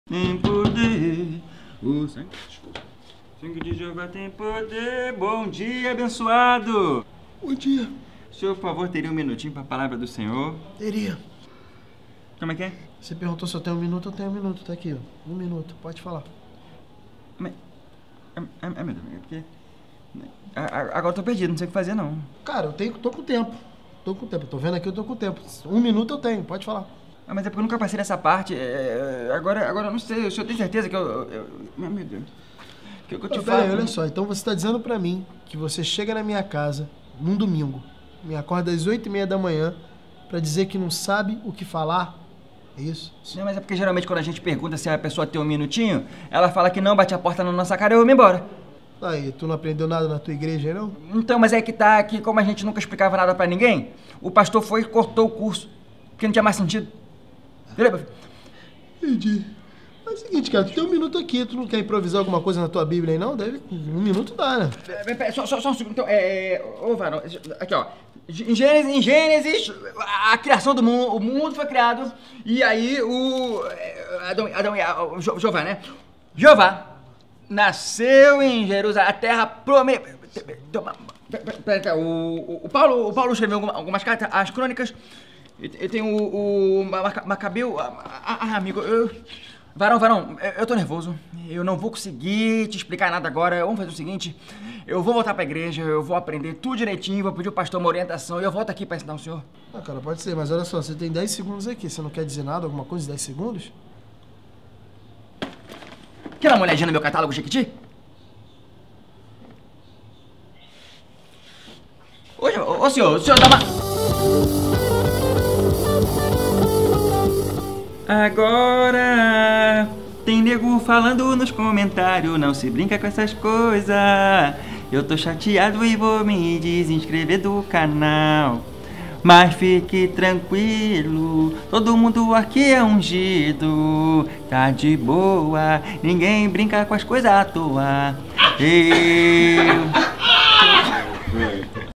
SÓ UM MINUTO
Todo mundo conhece eles... Aparecem aos domingos pela manhã para conversar com a gente por um minuto sobre o nosso bem e nunca paramos pra ouvir. Mas o que aconteceria se escutássemos? Você já testou?

ALAN RIBEIRO: PREGADOR
ISAÚ JUNIOR: MORADOR

DIREÇÃO: BRUNO HEITOR
PRODUÇÃO: CANAL IXI
ROTEIRO: CANAL IXI
EDIÇÃO: BRUNO DA SILVA COSTA